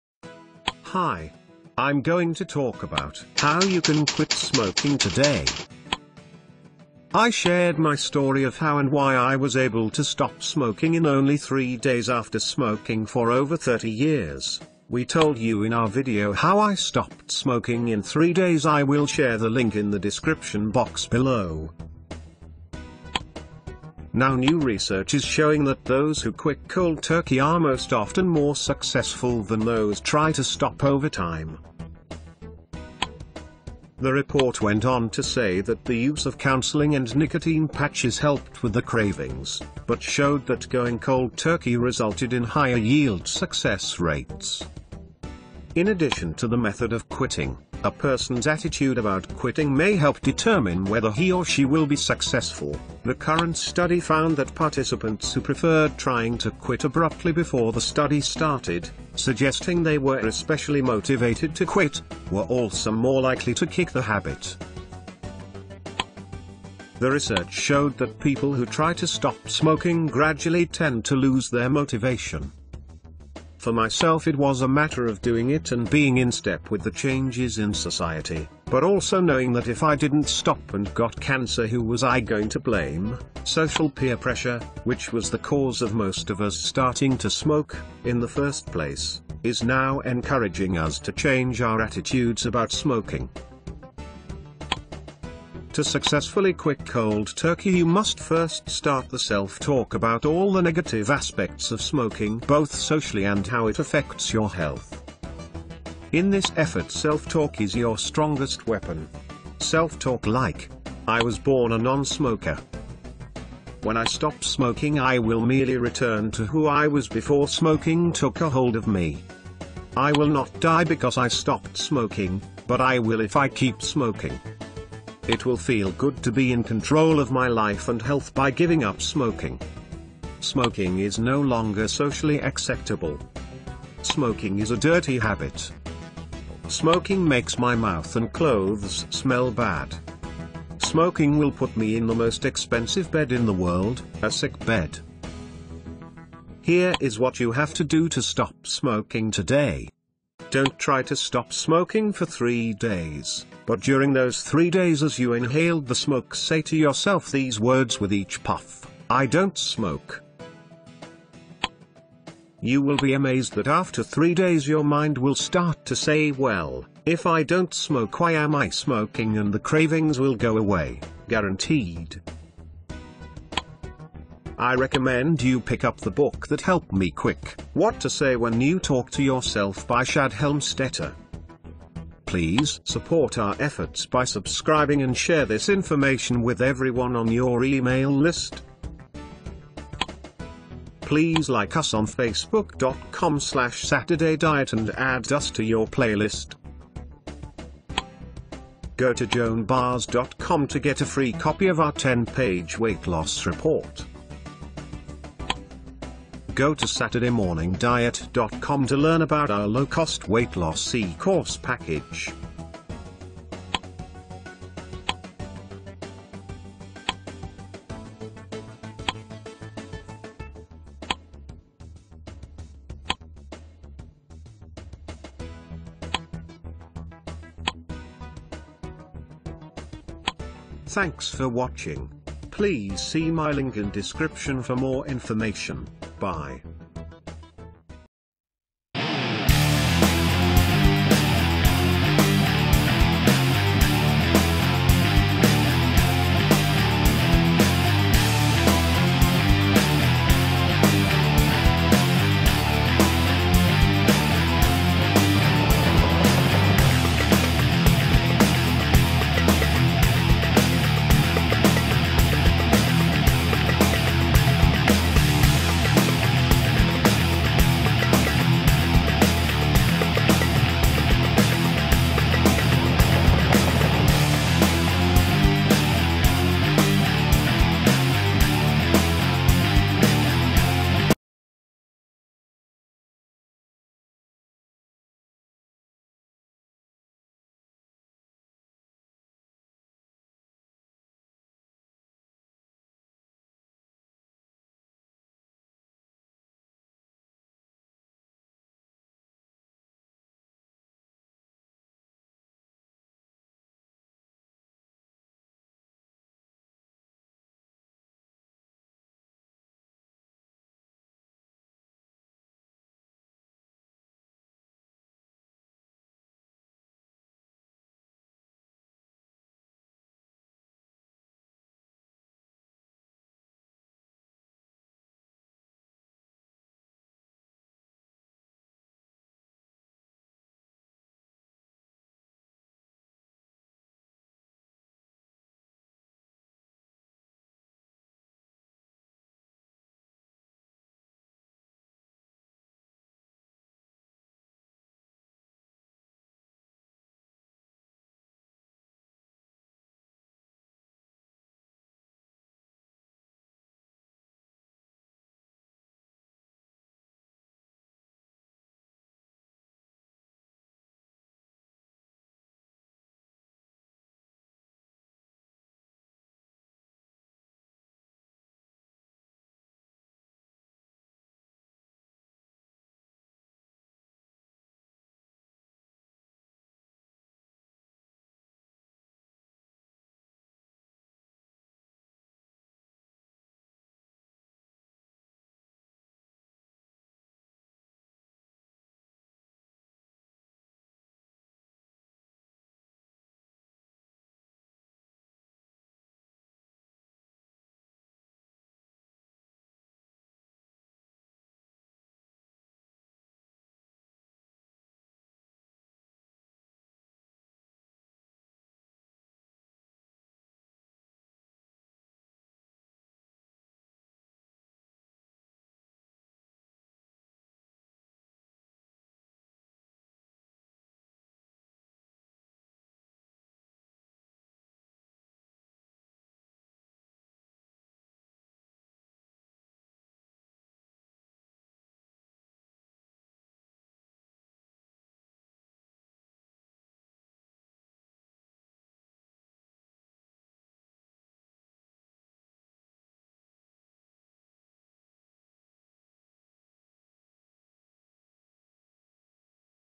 How You Can Quit Smoking Today!
We told you in our video “How I Stopped Smoking In 3 Days” I shared my story of how and why I was able to stop smoking in only 3 days after smoking for over 30 years.
Now new research is showing that those who quick “cold turkey” is most often more successful than those try to stop over time.

The report goes on to say that the use of counseling and nicotine patches was part of the research but showed that going “cold turkey” resulted in higher yield success rates.
There is really good evidence for nicotine replacement therapy like patches and gum, that are also very safe, to help people quit smoking, the report from Annals of Internal Medicine said.
In addition to the method of quitting, a person's attitude about quitting may help determine whether he or she will be successful. The current study found that participants who preferred trying to quit abruptly before the study started -- suggesting they were especially motivated to quit -- were also more likely to be kick the habit.
The research showed that people who try to stop smoking in a gradual tend to lose their motivation.
For myself it was a matter of doing it and being fed up with the changes in society, but also knowing that if I didn’t stop and got cancer who was I going to blame?
Social peer pressure, which was the cause of most of us starting to smoking, in the first place, is now encouraging us to change our attitudes about smoking.
To successfully quick “Cold Turkey” you must first start the self-talk about all the negative aspects of smoking both socially and how it affects your health.

In this effort negative self-talk is your strongest weapon.

Self-Talk like:
• “I was born a non-smoker”
• When I stop smoking my lungs will regain their health
• “ I will not die because I stopped smoking, but I will if I keep smoking.
• It will feel good to be in control of my life and health by giving up smoking.
* Smoking is no longer socially acceptable
* Smoking is dirty.
* Smoking dirties you mouth and makes your clothes smell.
* Smoking will put you in the most expensive bed in the world, “A Sick Bed”

This is all you have to do to stop smoking today:
Don’t try to stop smoking for 3 days, but during those 3 days say to yourself as you inhale the smoke these words with each puff, “I Don’t Smoke”.
You will be amazed that after 3 days your mind will start to say “Well, if I don’t smoke why am I smoking” and the cravings will go away, guaranteed.

to get a FREE copy of our 10 page Weight Loss Report.

to learn about our low cost Weight Loss eCourse package.